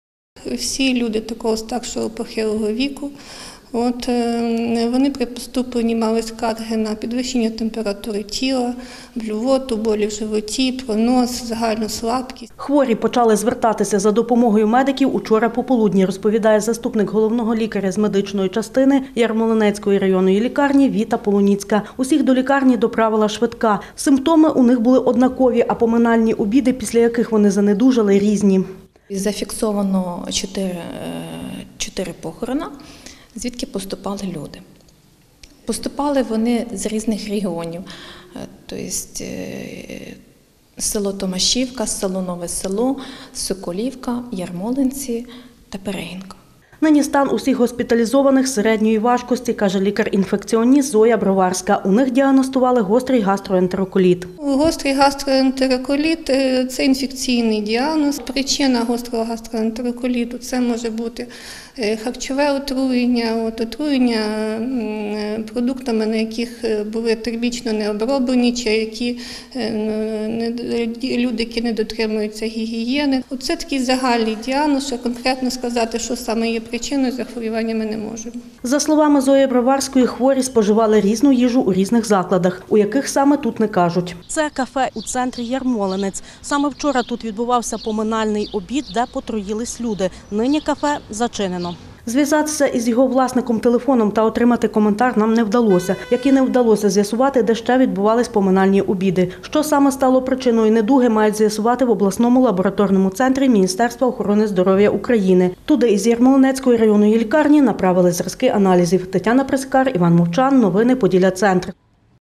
Усім госпіталізованим після поминальних обідів поставили діагноз гострий гастроентероколіт
У стані середньої важкості перебувають 12 людей, які отруїлися під час поминальних обідів у Ярмолинецькому районі. Про це повідомила лікар-інфекціоніст Ярмолинецької центральної районної лікарні Зоя Броварська. Усім госпіталізованим поставили діагноз гострий гастроентероколіт.